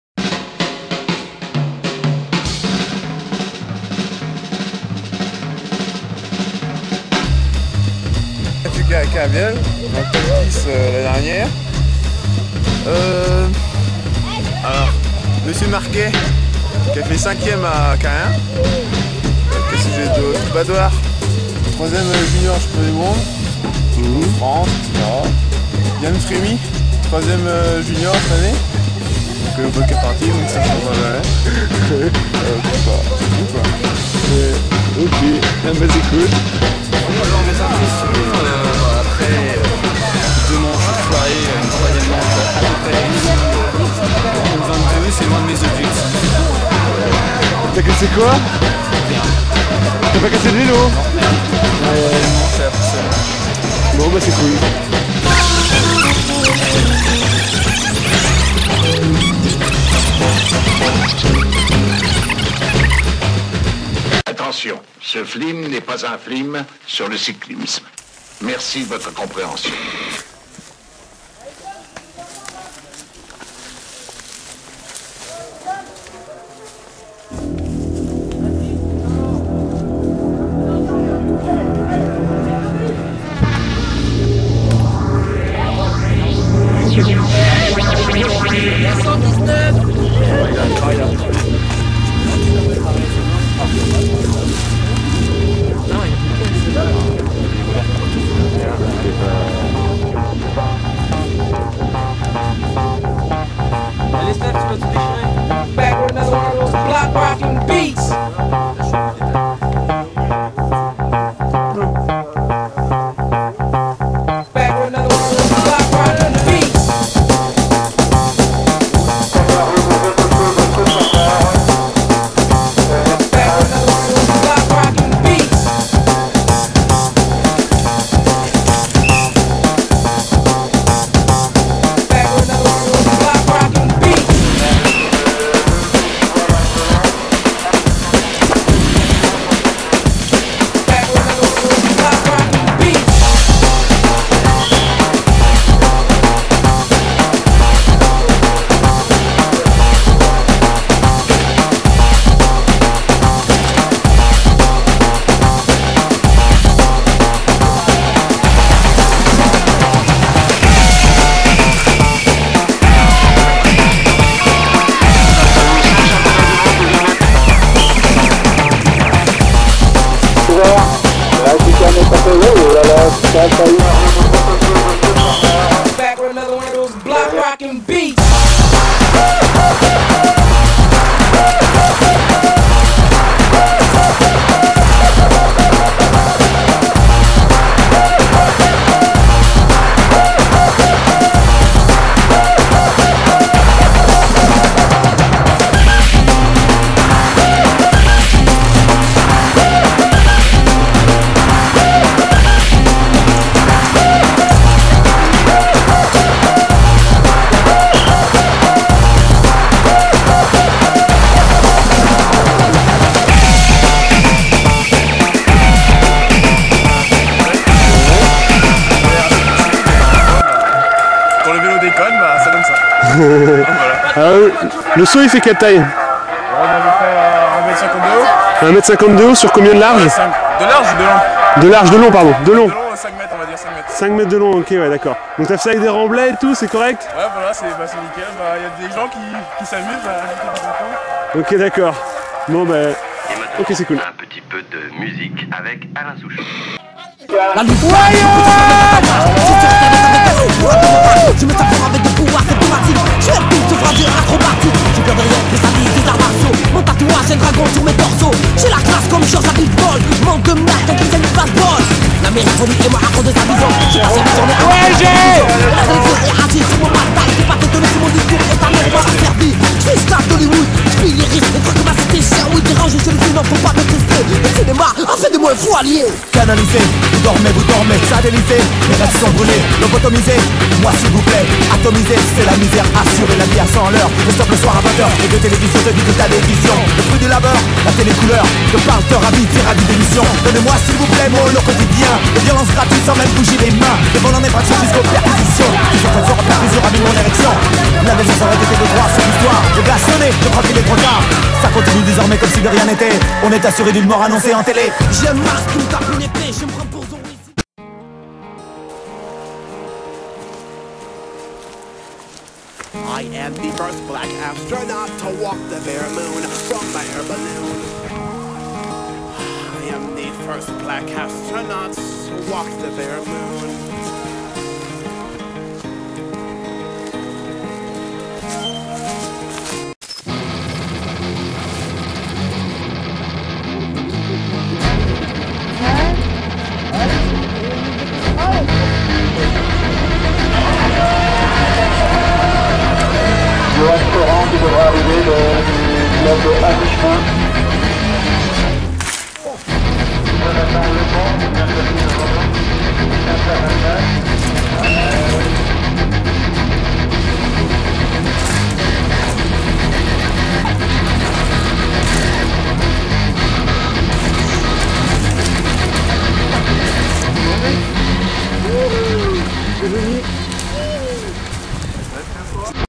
5 ième manche de la coupe de Bretagne 2007
Une petite video sur la 5 ième manche de la coupe de Bretagne de DH 2007 organisé par le Kemperle Yaouankiz Team